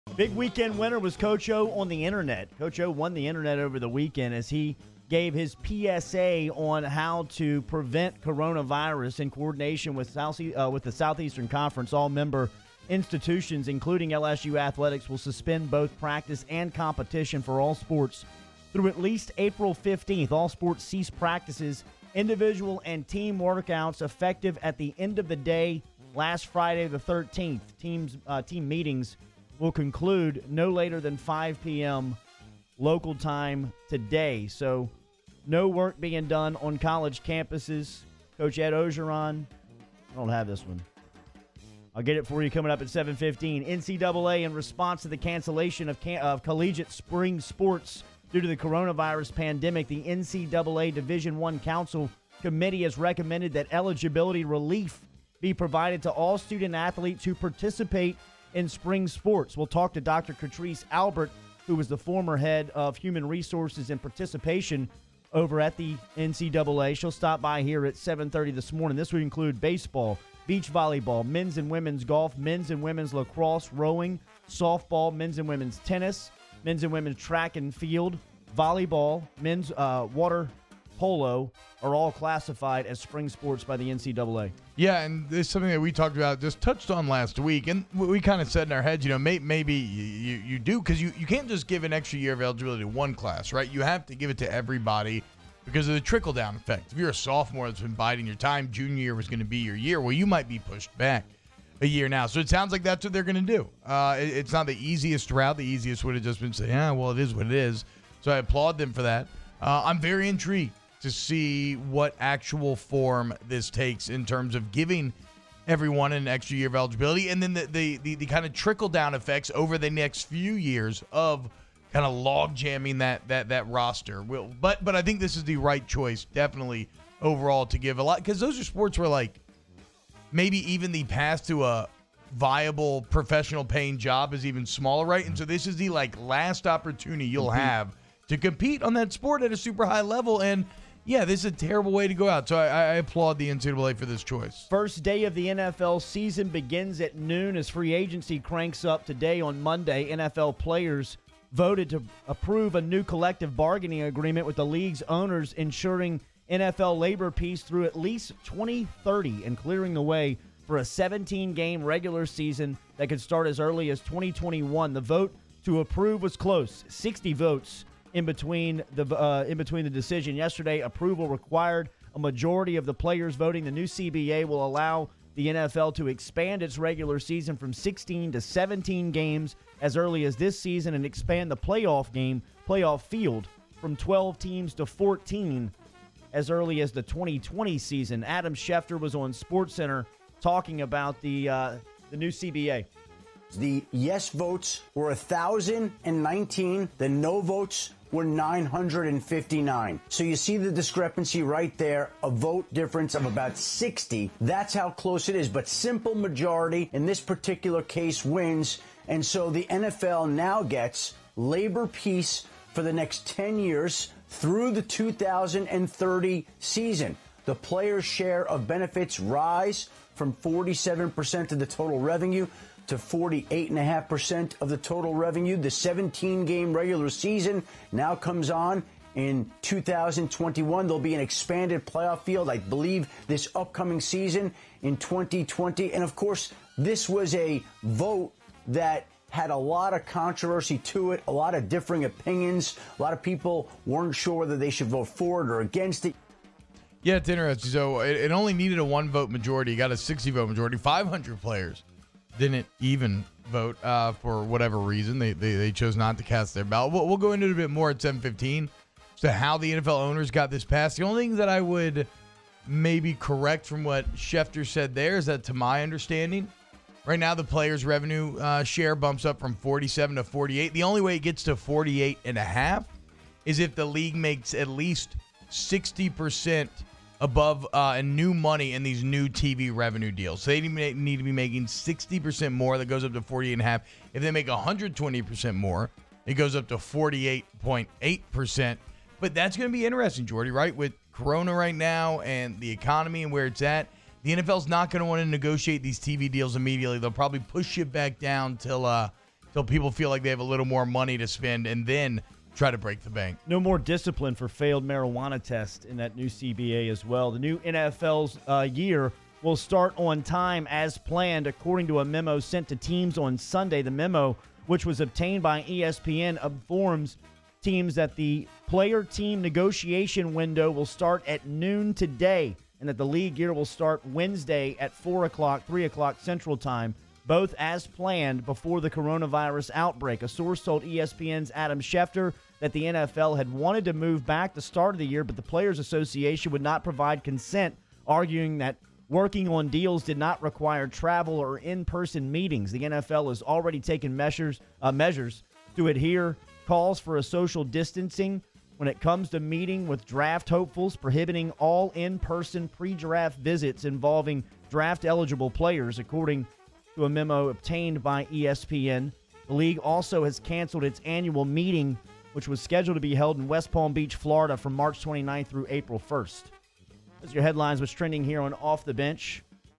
Headlines - LSU Practice Suspended, NFL Players Approve CBA
LSU Sports
In coordination with the Southeastern Conference, all member institutions, including LSU Athletics, will suspend both practice and competition for all sports through April 15.
All sports ceased practices, individual and team workouts effective the end of the day Friday, March 13. Team meetings will conclude no later than 5 pm local time today.

NCAA
In response to the cancellation of collegiate spring sports due to the coronavirus pandemic, the NCAA Division I Council Committee has recommended that eligibility relief be provided to all student-athletes who participate in spring sports.
"Details of eligibility relief will be finalized at a later time," the NCAA said Friday in a statement. "Additional issues with NCAA rules must be addressed, and appropriate governance bodies will work through those in the coming days and week."
Baseball, beach volleyball, men's and women's golf, men's and women's lacrosse, rowing, softball, men's and women's tennis, men's and women's track and field, men's volleyball and women's water polo are all classified as spring sports by the NCAA.

NFL Collective Bargaining Agreement
NFL players voted to approve a new collective bargaining agreement with the league's owners, ensuring NFL labor peace through at least 2030 and clearing the way for a 17-game regular season as early as 2021.
The vote to approve was close, at 1019-959. Approval required a majority of the players voting.
The new CBA will allow the NFL to expand its regular season from 16 to 17 games as early as 2021 and expand the playoff field from 12 teams to 14 as soon as the 2020 season.
It also includes higher minimum salaries, improvements to benefits for current and former players, expanded rosters and practice squads, and changes to the league's drug and discipline policies, all of which will go into effect in 2020.

NFL
The NFL's new league year will start on time as planned this week, according to a memo sent to teams Sunday.
The memo, which was obtained by ESPN, informs teams that the player-team negotiation window will start at noon ET on Monday and that the league year will start Wednesday at 4 pm ET -- both as were planned before the coronavirus outbreak.
A source told ESPN's Adam Schefter that the NFL had wanted to move back the start of the year, but the players' association would not provide consent, arguing that working on deals did not require travel or in-person meetings.
The NFL already has taken measures to adhere to calls for social distancing when it comes to meeting with draft hopefuls, prohibiting "all in-person, pre-draft visits involving draft-eligible players," according to an earlier memo obtained by ESPN's Dan Graziano.
The league also has canceled its annual meeting, which was scheduled to be held in West Palm Beach, Florida, from March 29 through April 1.